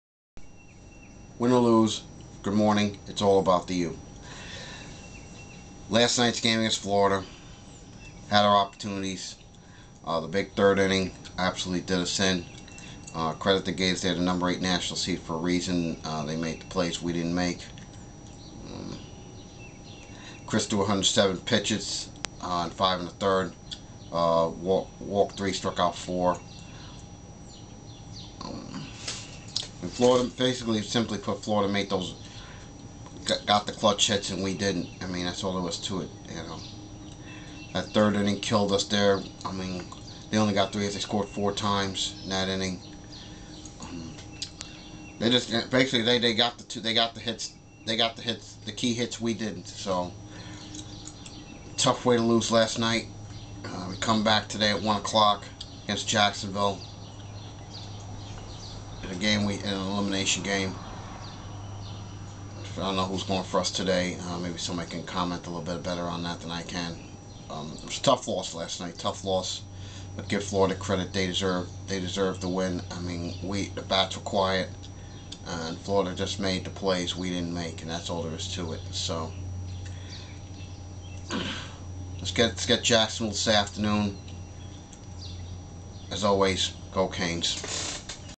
Miami Hurricanes vs Florida Gators recap from 5/30/09
My recap of the CANES 8-2 loss to Florida in the Winner's Bracket of the Gainesville Regional on 5/30/09.